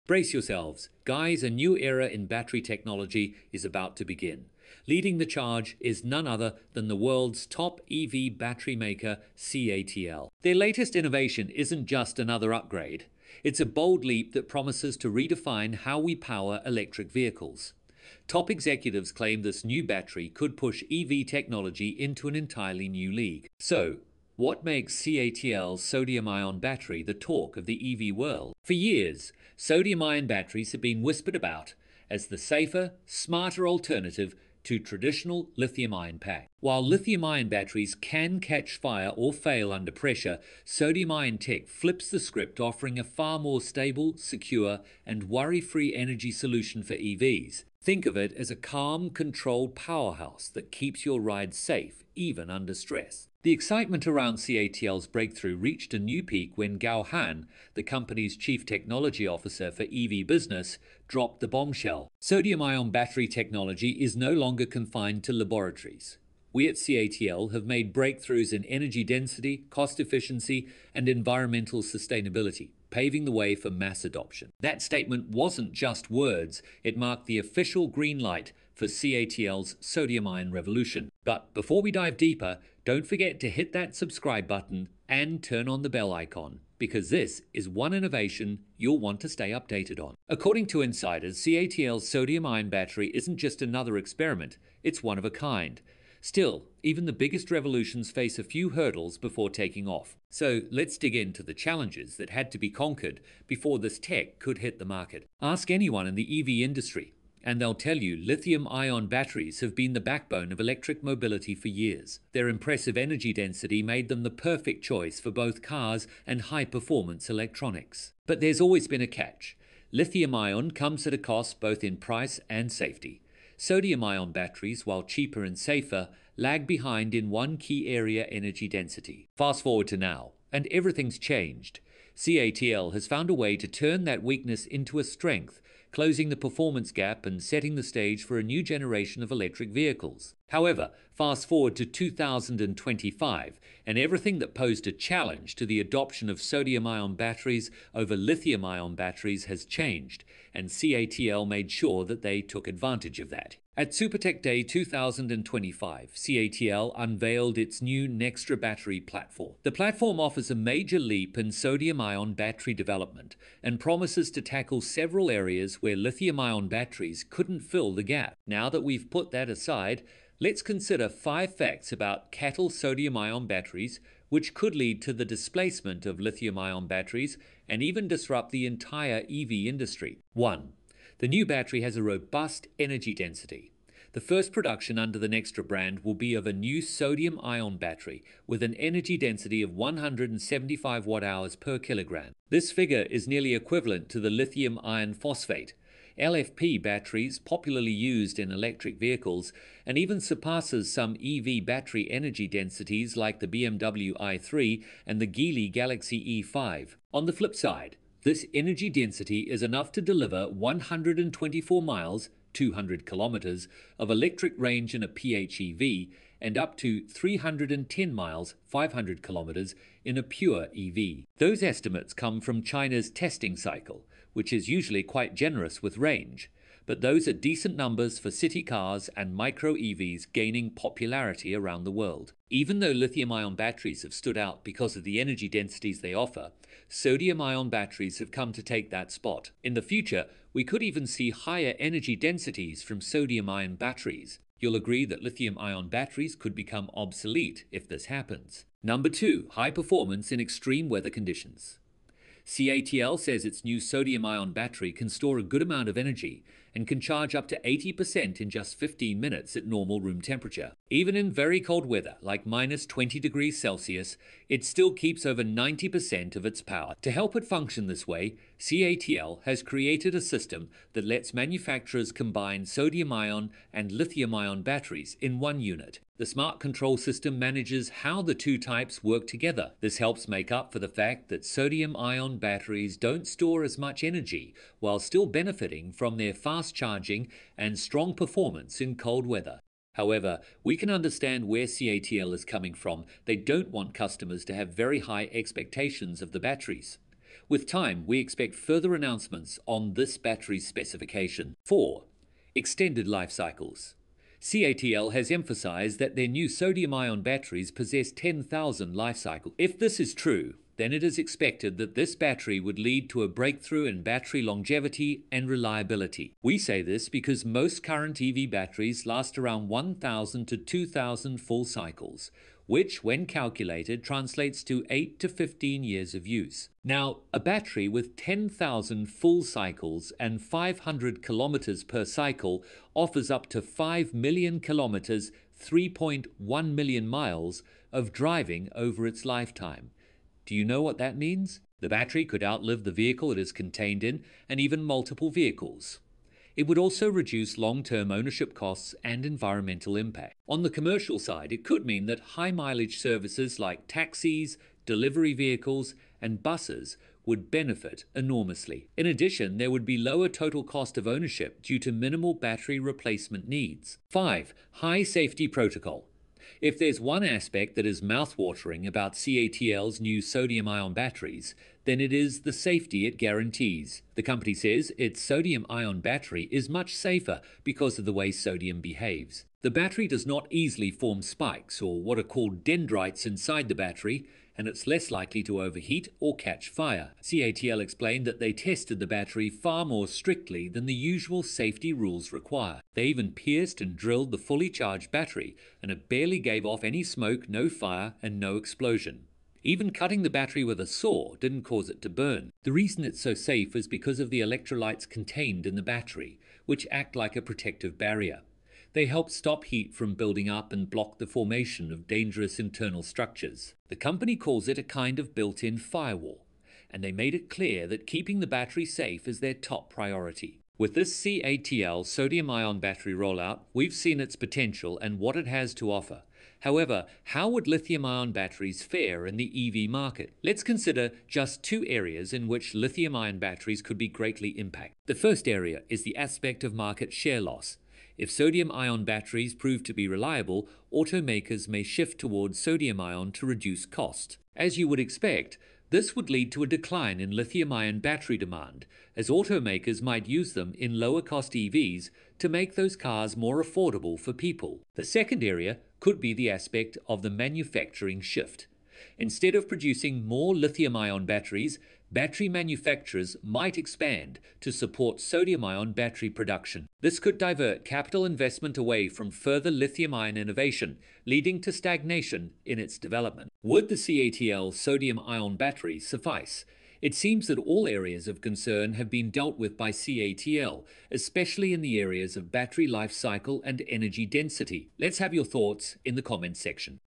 CATL launched a new sodium battery with a 5-million-kilometer lifespan that will take over lithium
CATL launched a new sodium battery with a 5-million-kilometer lifespan that will take over lithium batteries.

The new battery about to be launched has been said by top executives to take the EV battery technology to the next level. What is it about the C-A-T-L Sodium-ion battery that has made it gain attention in the EV battery space?

For many years, Sodium-ion batteries have long been pointed to as a safer alternative to the common lithium-ion batteries used in many EVs. In contrast to the use of lithium-ion batteries, which carry a risk of fire when it has been subjected to damage or force, sodium-ion batteries do the opposite. Yes, it offers a more stable and secure energy storage solution for EVs and guarantees safety to a large extent.

The potential of C-A-T-L was highlighted by C-A-T-L Chief Technology Officer for EV Business, Gao Huan during a recent announcement. In his words he said;
“Sodium-ion Battery technology is no longer confined to laboratories. We at C-A-T-L have made breakthroughs in energy density, cost-efficiency, and environmental sustainability, paving the way for mass adoption.”

This announcement by Gao opened the way for the release of the C-A-T-L sodium-ion battery technology.

This technology has been announced by several stakeholders at C-A-T-L to be one of a kind. However, several challenges were left to be overcome before its release. Let's examine that.

Those in the EV manufacturing industry would tell you that Lithium-ion batteries have been the industry’s cornerstone since the industry gained acceptance. This is because lithium-ion batteries have an impressive energy density. This high energy storage capability per unit volume makes these batteries ideal for applications like electric vehicles and high-performance electronics. On the other hand, Sodium-ion batteries, with their inherent lower energy density were not favorites to be used as EV batteries even when the price of Lithium-ion batteries dropped.
However, fast forward to 2025, and everything that posed a challenge to the adoption of sodium-ion batteries over Lithium-ion batteries has changed, and C-A-T-L made sure that they took advantage of that.
At Super Tech Day 2025, C-A-T-L unveiled its new Naxtra Battery platform. The platform offers a major leap in sodium-ion battery development and promises to tackle several areas where lithium-ion batteries couldn't fill the gap.

Now that we've put that aside, let's consider five facts about C-A-T-L sodium-ion batteries, which could lead to the displacement of lithium-ion batteries and even disrupt the entire EV industry.